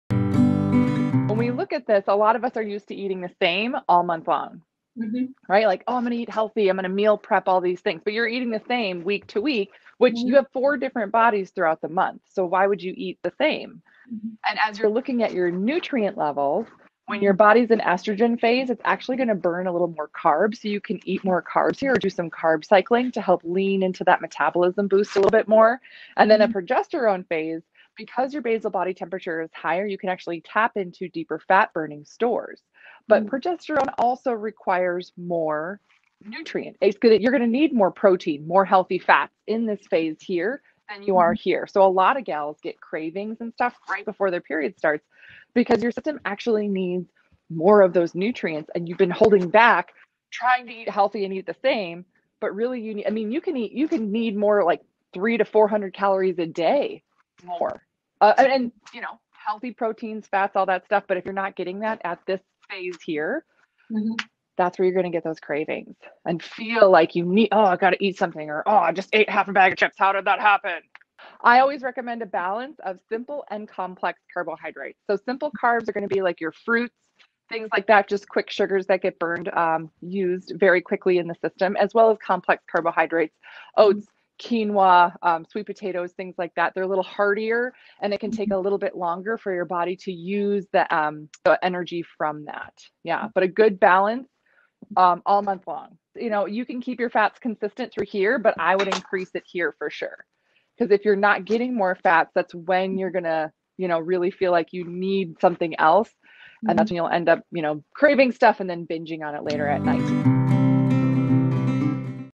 Female Menstrual Cycle
Learn about Menstrual Cycle from Dr. Beth Westie.
Menstrual cycle is the series of monthly menstruation which occurs in a female’s body.
It is the process which takes place in preparation for the possibility of pregnancy.
An ovum(egg) is released by an ovary and this is called ovulation, at the same time the uterus prepares itself by preparing a thick layer for protection of the zygote after fertilization.
But if the fertilization does not take place the lining of the uterus sheds off with blood through vagina.
This process may develop some complications which need to be solved immediately.
Know about what to eat during mensuration, and how to handle the complications from our expert!